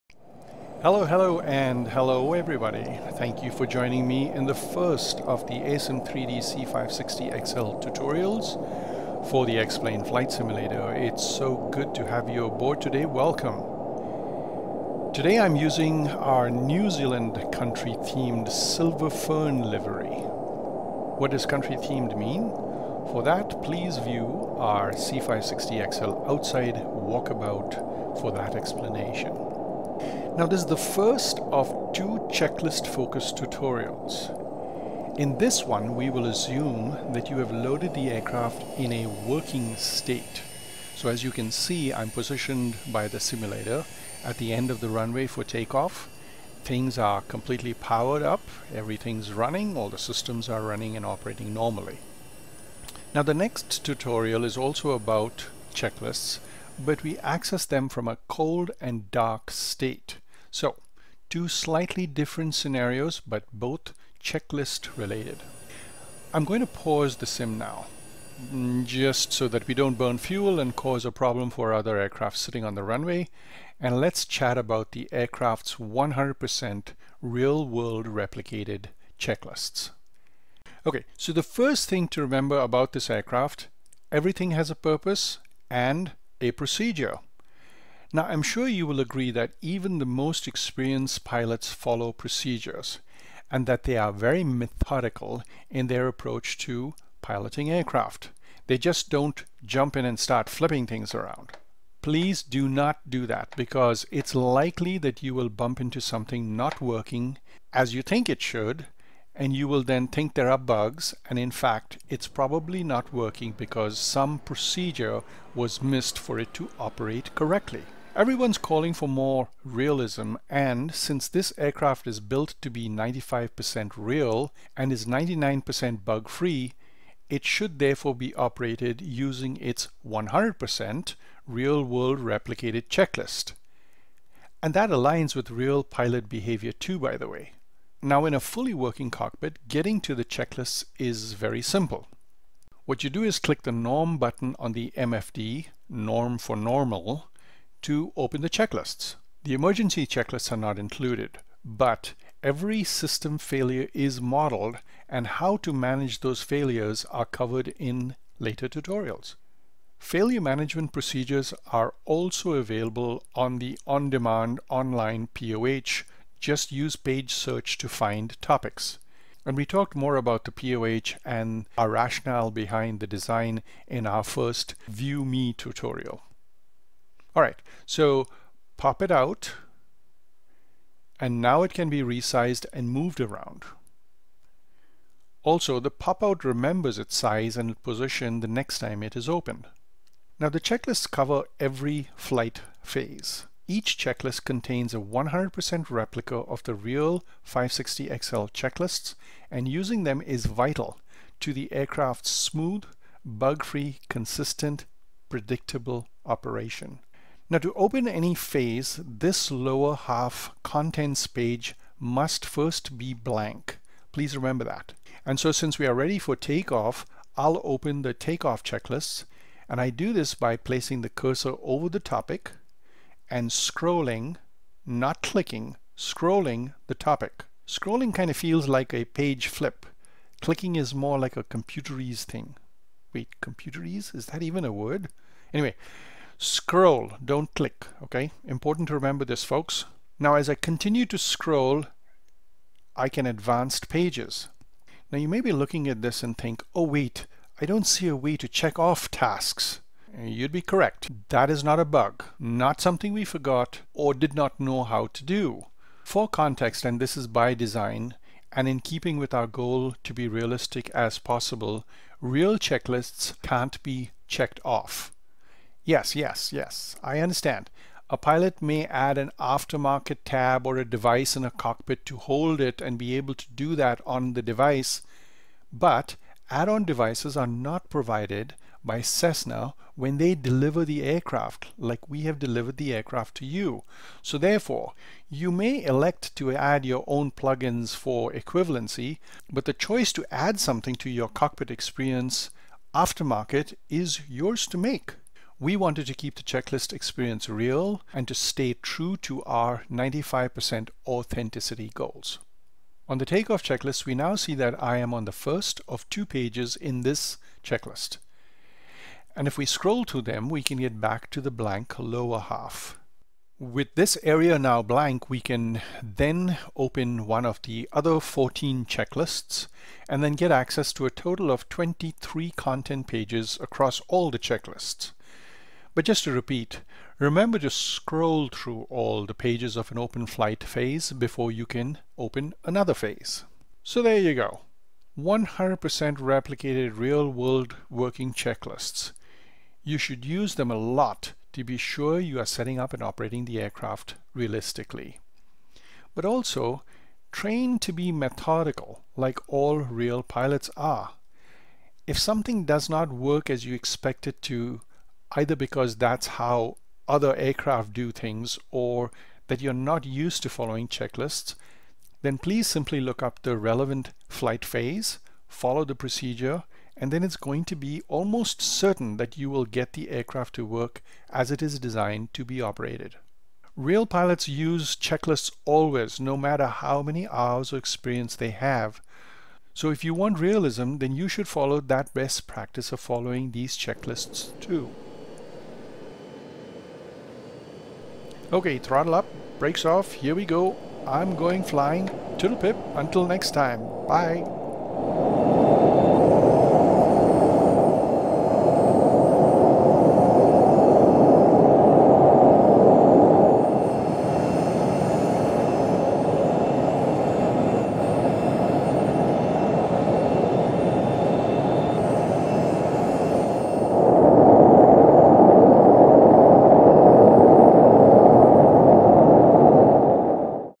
AirSim3D Citation C-560XL Tutorial #1.1: Checklists (Part1)
This is the first of 2 Checklists Tutorials.

Methodical checklist use is vital to the smooth, predictable, reliable and frustration-free operation of this highly replicated aircraft!  Every real pilot uses them, always, no matter their experience. And if you want realism, then you should make using this aircrafts' checklists part of your flight routine (well... at LEAST for enough time to get really familiar with its procedures), then once learned, the aircraft is easy to fly -- very forgiving, and super responsive too -- but not in a twitchy way.  Solid. Predictable. (actually that is partly because the model uses real-world, customized airfoils!)

In this tutorial, we access the checklists in a fully working, up and running cockpit, and we learn how to use them, change topics etc. -- and no they are not click-to-open! So please don't skip it.

In the next Part 2, we assume you've seen Part 1, as we access Checklists in a true Cold and Dark cockpit state.